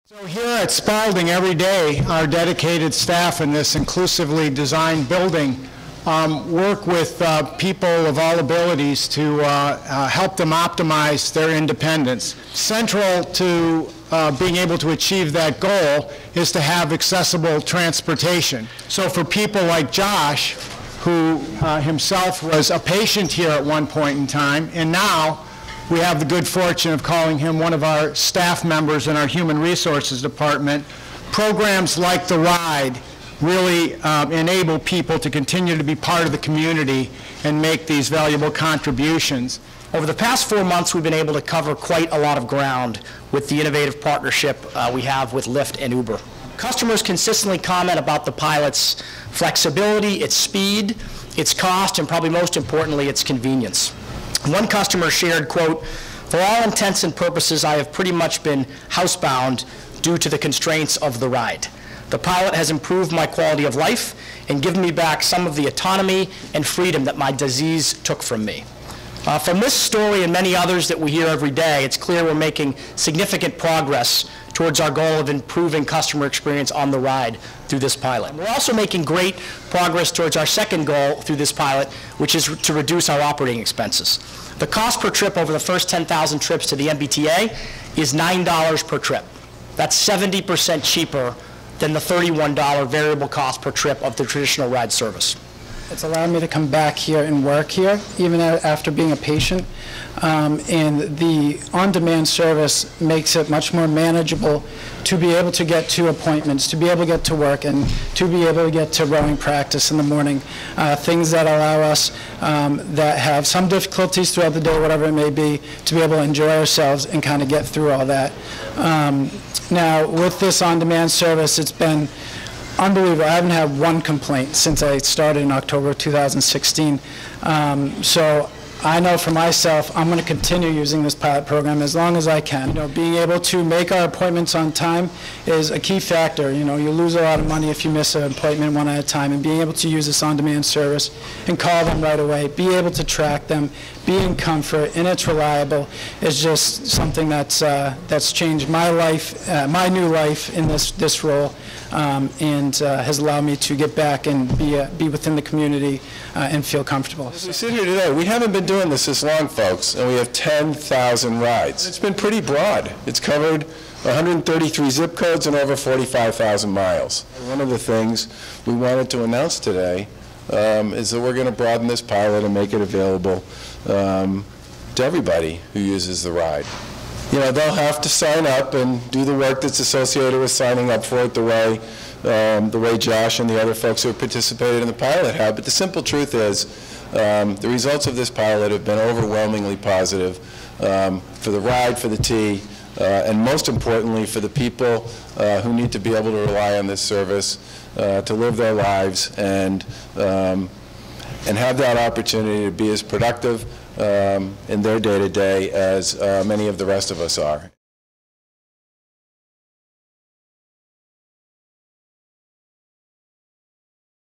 Governor Baker, MBTA Celebrate Expansion of The RIDE’s On-Demand Paratransit Service
Governor Charlie Baker and MBTA Acting General Manager Brian Shortsleeve celebrated 10,000 rides in the MBTA’s The RIDE On-Demand Paratransit Pilot Program with ride-share companies Uber and Lyft, and announced that effective March 1st, the program will be open to all eligible users of The RIDE. Launched in September 2016, the first-of-its-kind innovative pilot has expanded options for 400 customers with disabilities, providing improved flexibility and mobility while reducing fares and overall program costs.